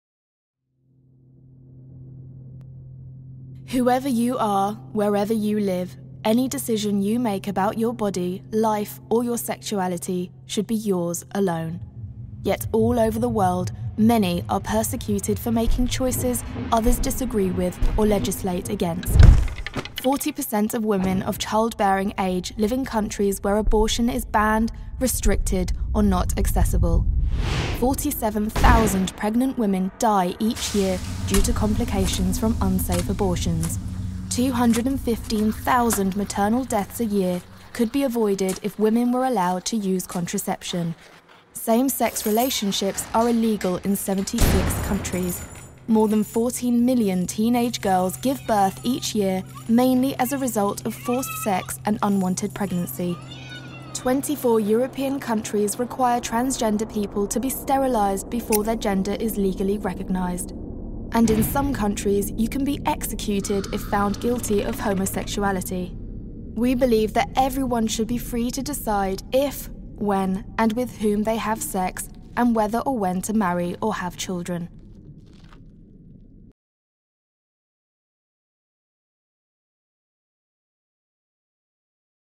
Il mio corpo: i miei diritti
I diritti sessuali e riproduttivi in diversi paesi sono controllati da stato, operatori sanitari o dalla famiglia attraverso leggi e sanzioni penali. E' ora di dire basta!
Sottoscrivi il manifesto, chiediamo ai governi e alle autorità di lasciarci libere e liberi di decidere!